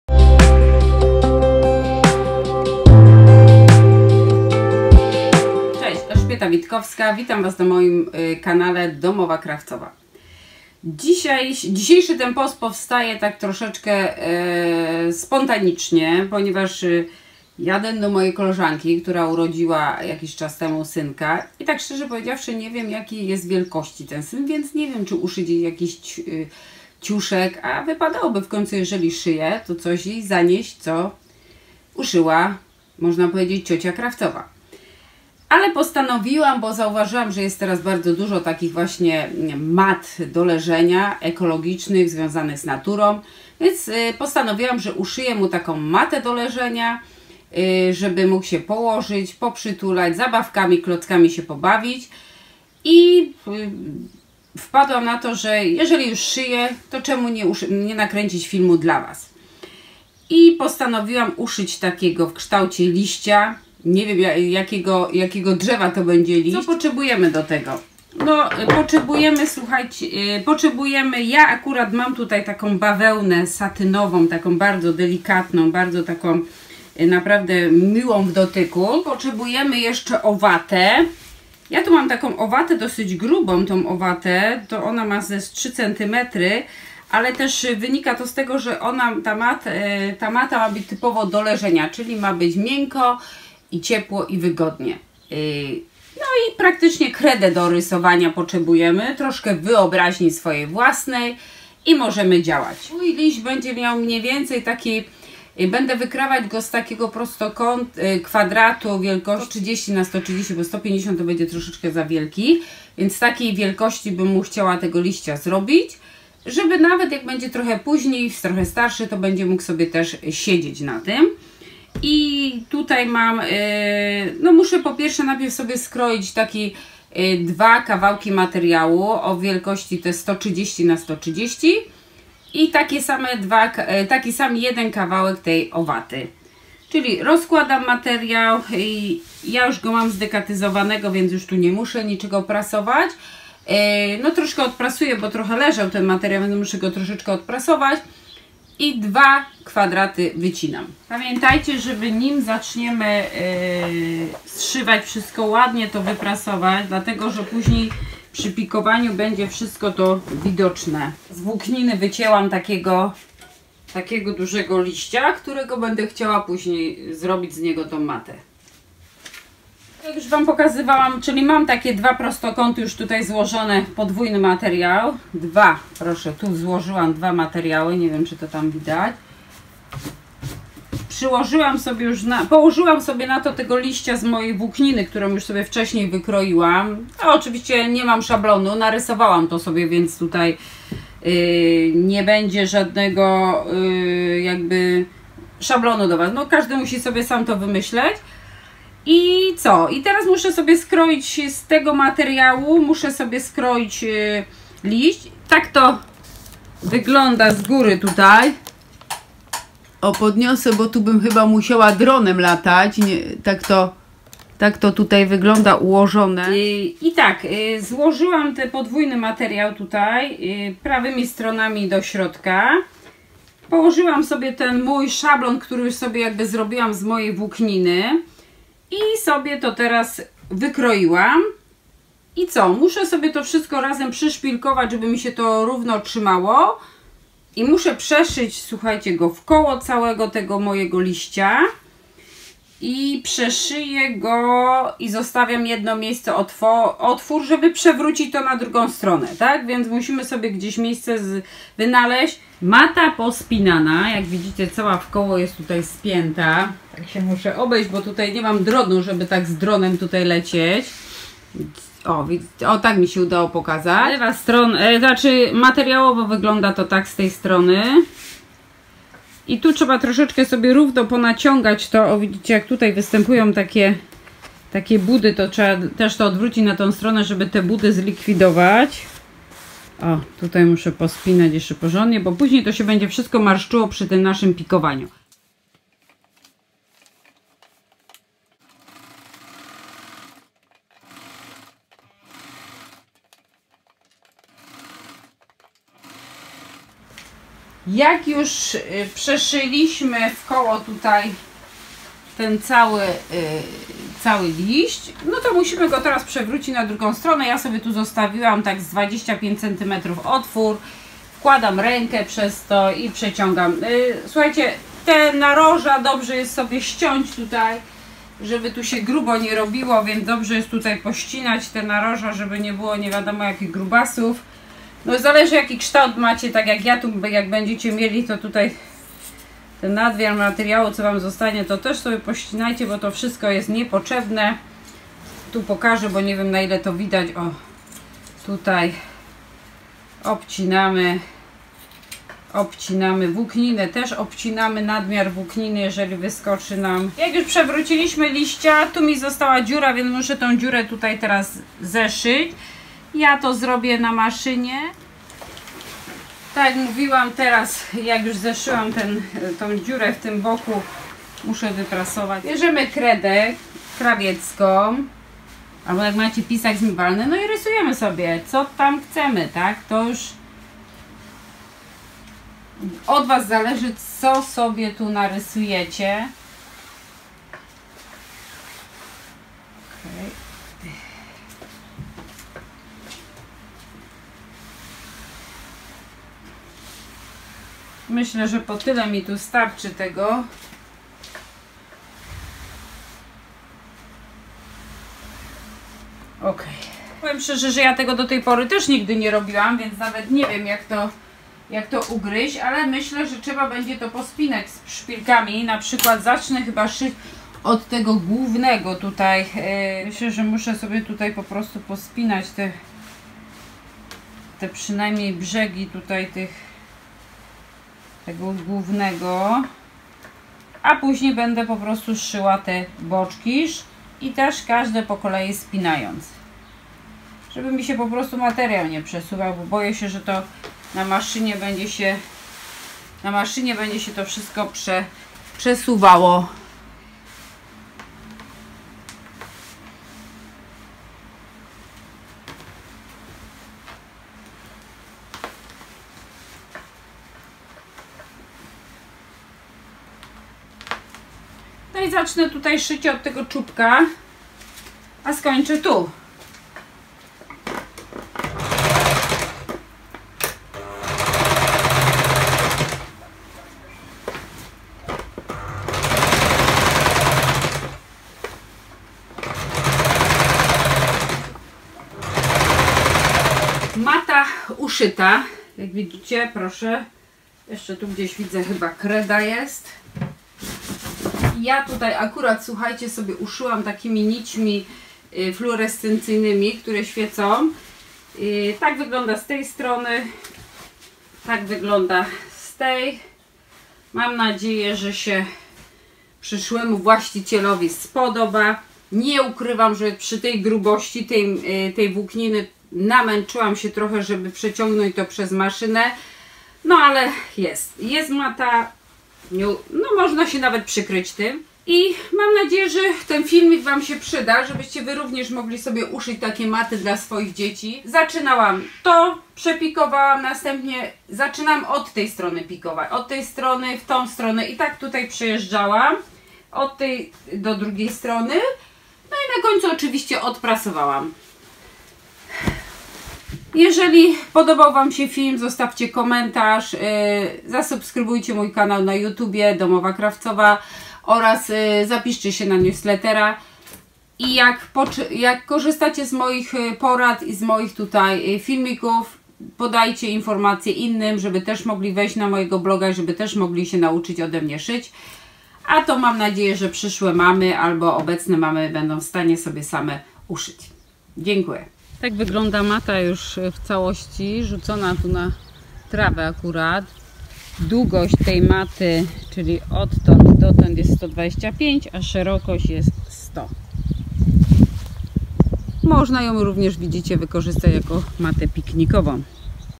Jak uszyć matę do leżenia dla dziecka
Zapraszam Cię do uszycia ze mną maty  do leżenia dla dziecka w kształcie liścia. Nie będę ukrywać, że troszkę się pomęczyłam grubościami warstw, ale było warto by zobaczyć uśmiech obdarowanego chłopca.
Więcej inspiracji znajdziesz na blogu :
Wykroje na ubrania, torebki lub zabawki znajdziesz w sklepie:
Zapraszam również na kursy szycia ubrań, torebek, i nakryć głowy.